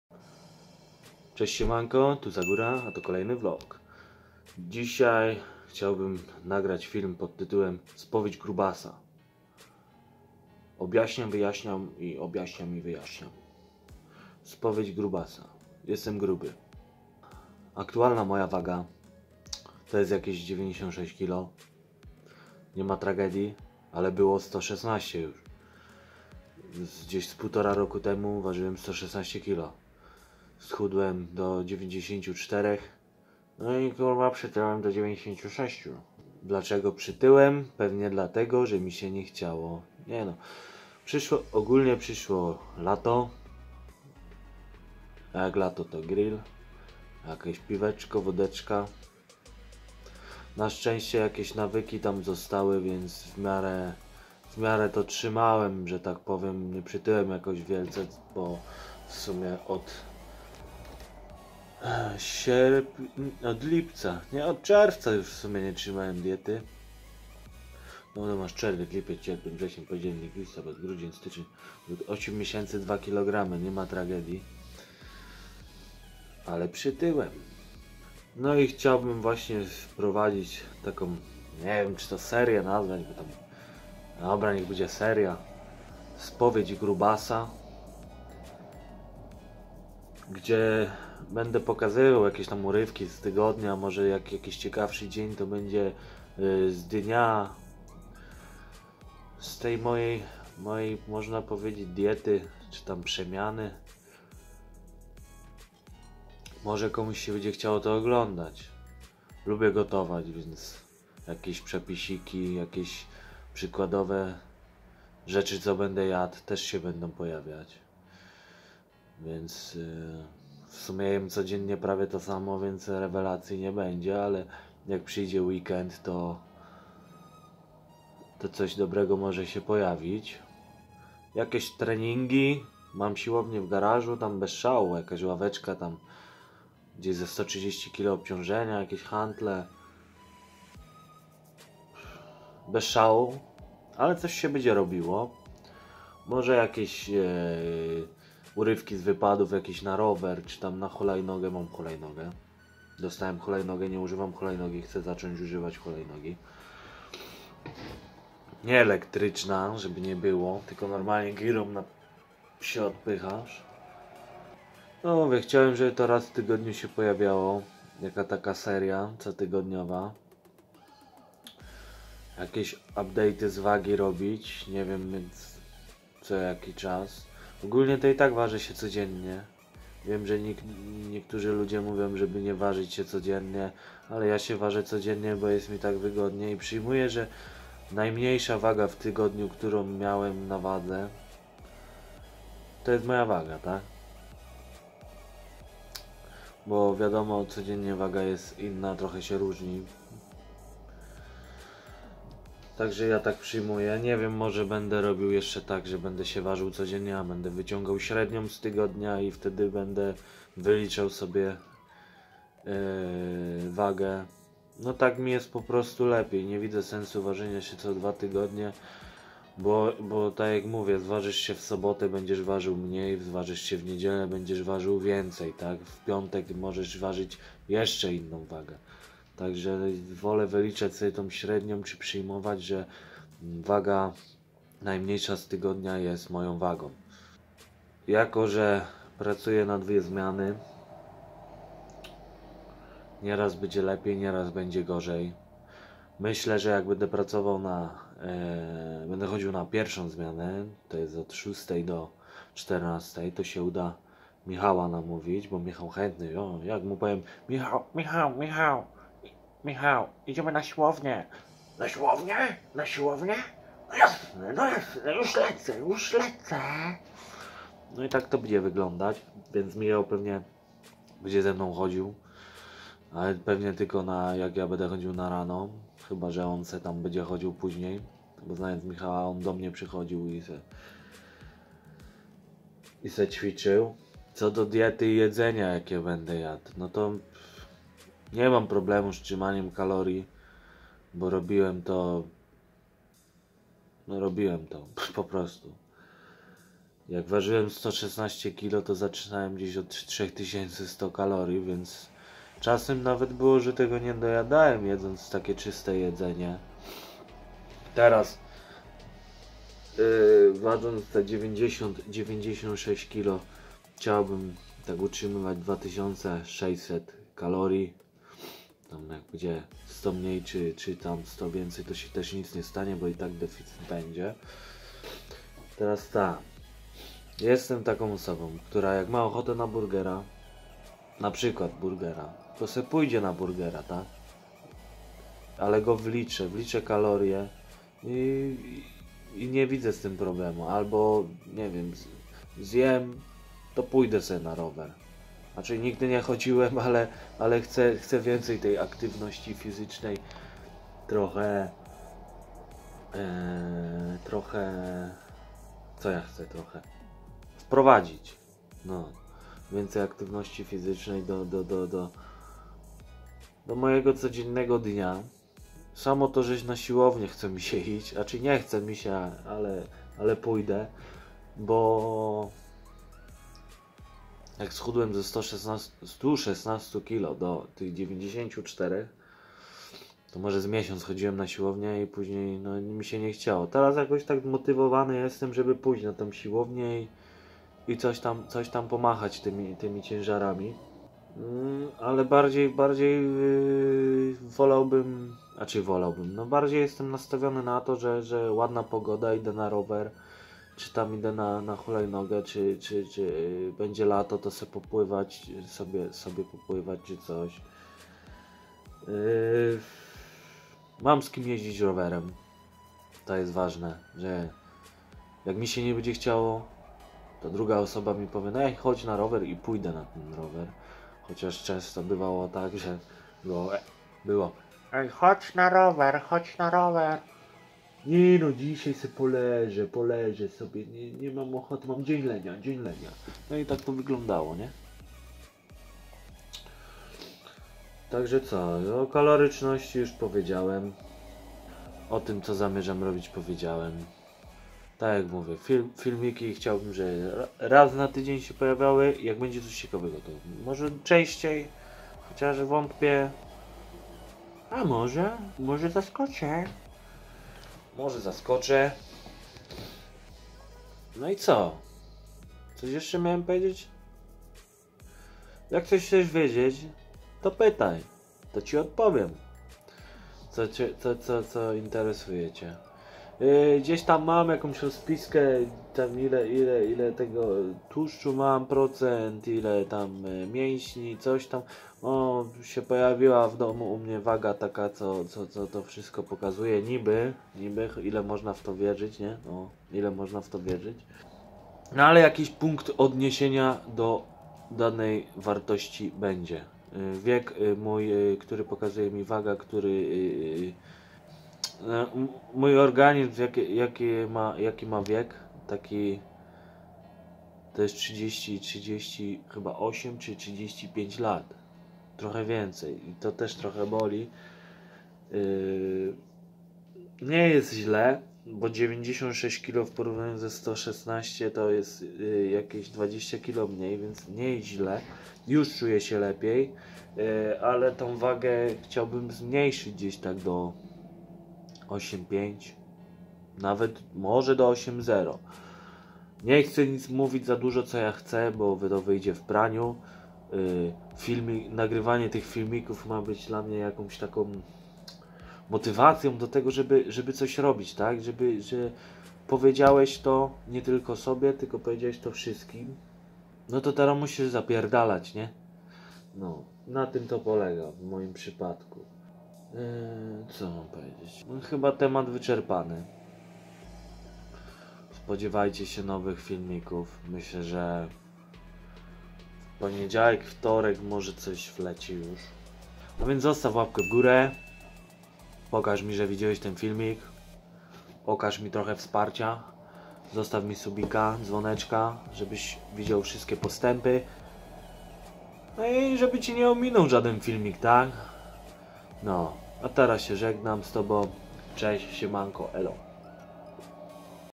Spowiedź grubasa ! Nowe postanowienie ! Vlog 1
Spowiedź grubasa seria w której podejmuje próbę zrzucenia wagi.
vlog z tej serii bedzie sie pojawiać conajmniej raz w tygodniu
Zapraszam do oglądania i zostawienia czegoś po siobie !